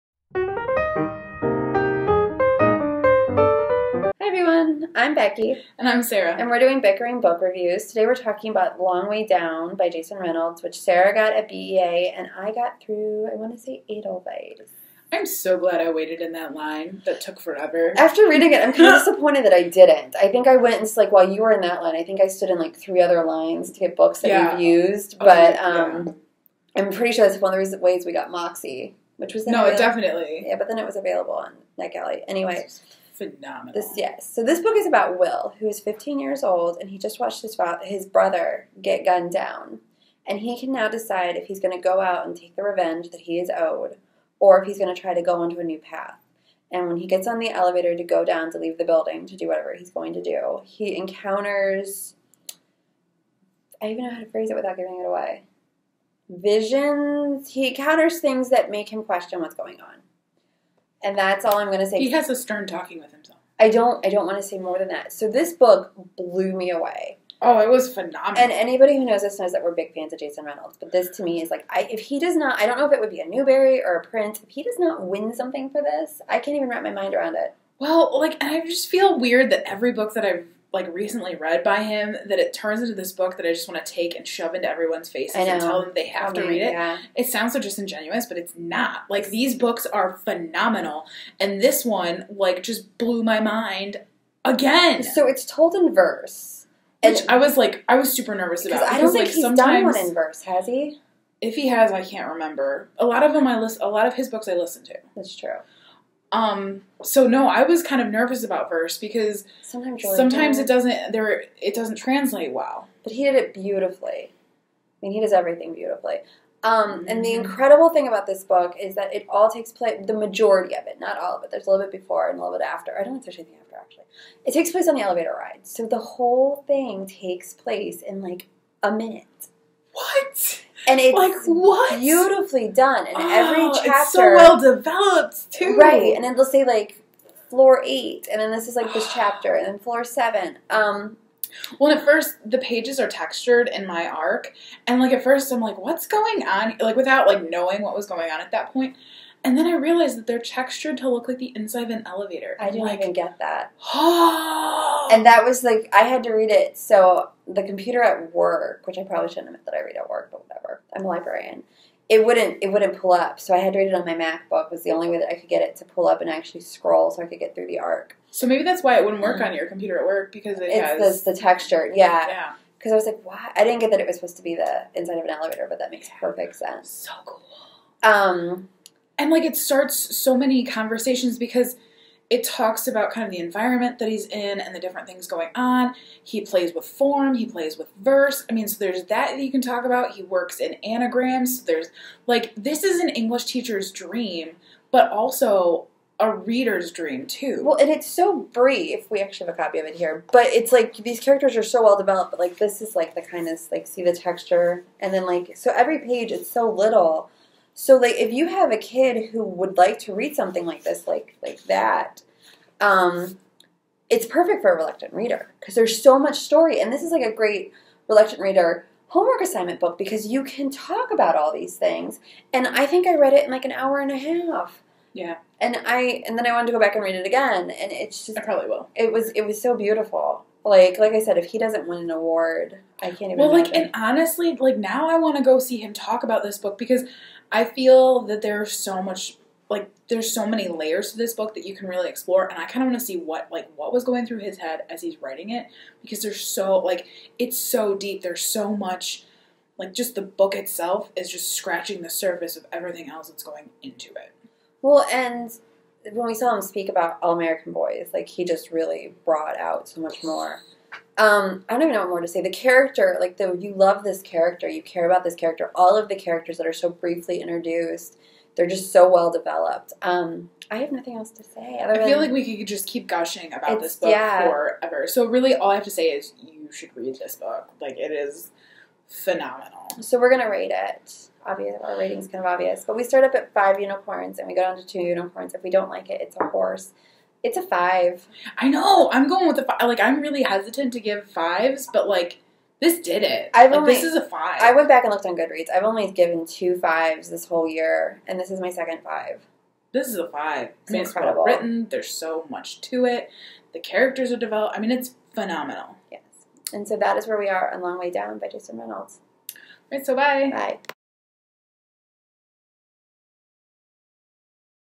Long Way Down by Jason Reynolds Book Review
Bickering Book reviews book review of "The Long Way Day" by Jason Reynolds.